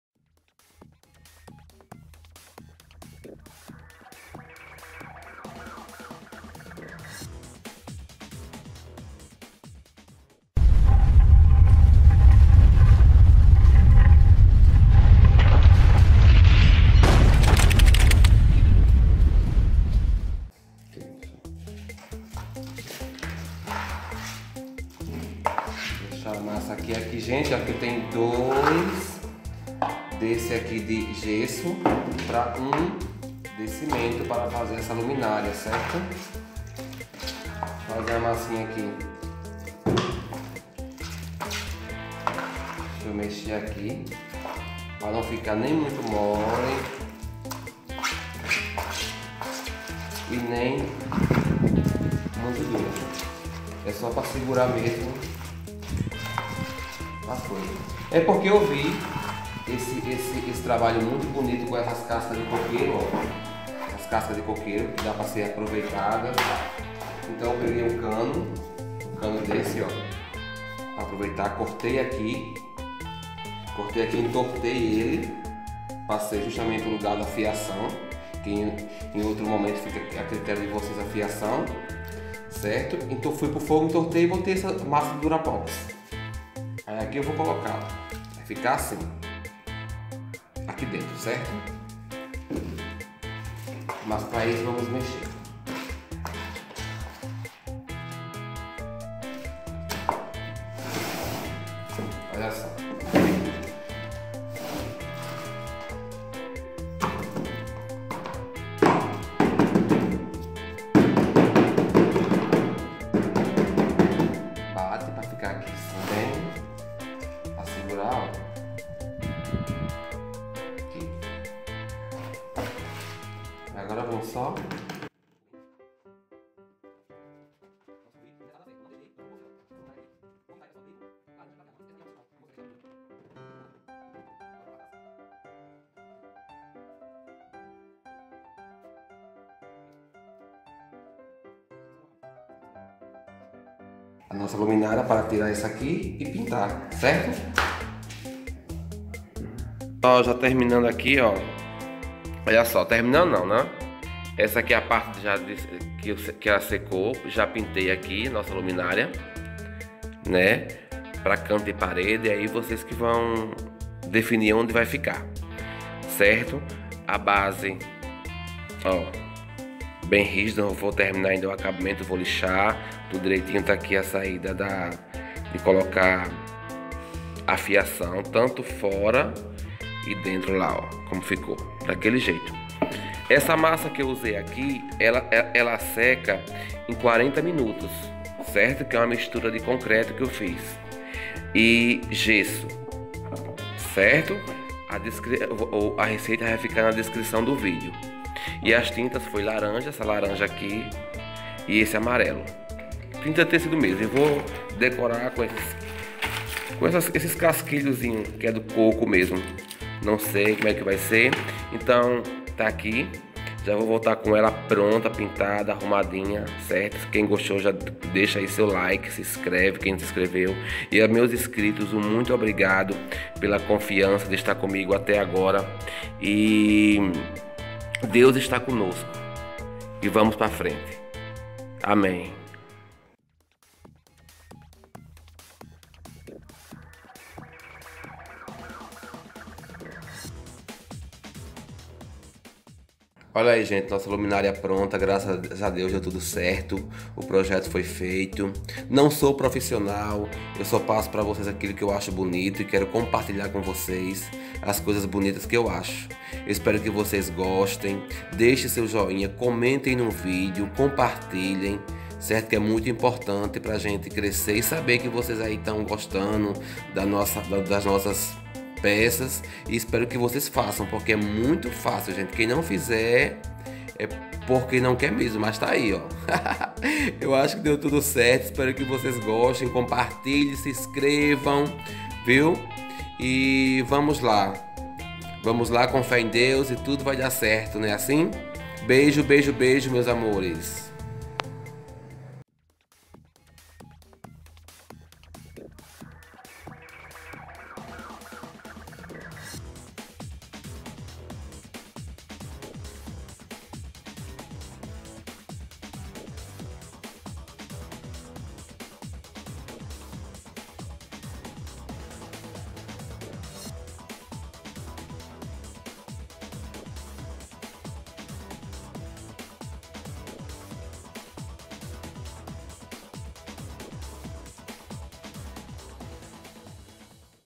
Luminária feita com casca de coqueiro
Olá pessoal!
Vamos fazer uma luminária feita com casca de coqueiro.
Iremos utilizar casca de coqueiro óbvio, um pedaço de cano, e a base sera feita de concreto e gesso.
Receita da massa:
500 g de gesso
250 g de cimento
adiciona água e mistura  bem.
Também vamos utilizar  tinta de tecido de várias cores para decorar a luminária, as cores fica a critério de vocês.
Bora lá fazer arte.